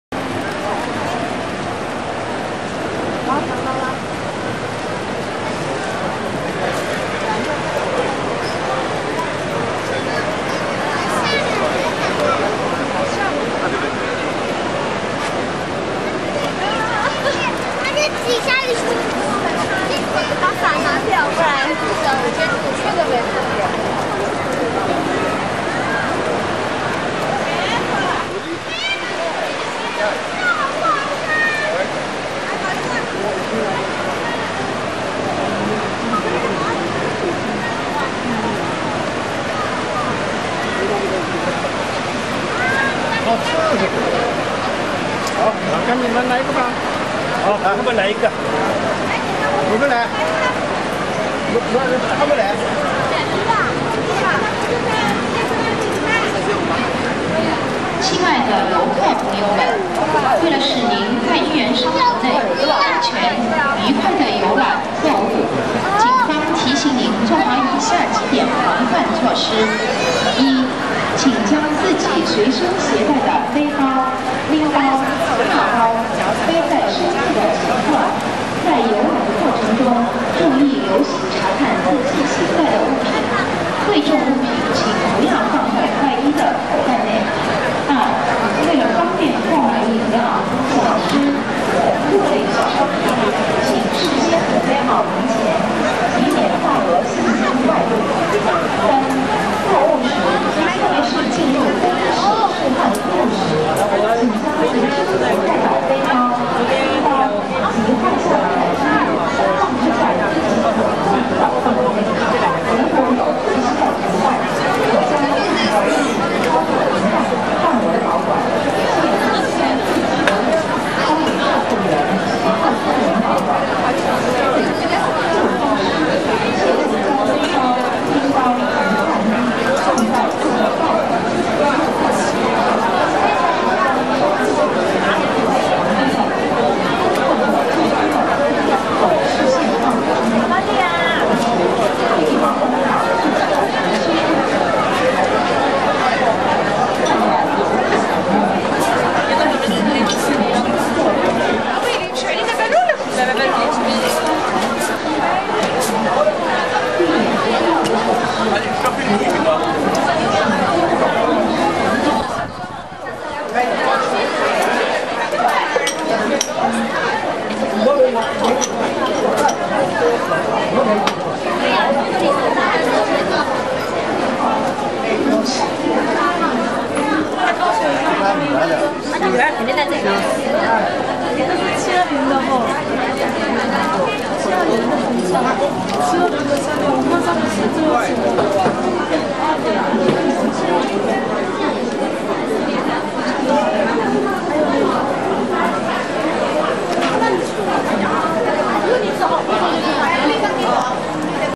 Shanghai Marketplace 2010 XI
Take a rainy day stroll through a historic market in Shanghai in July 2010.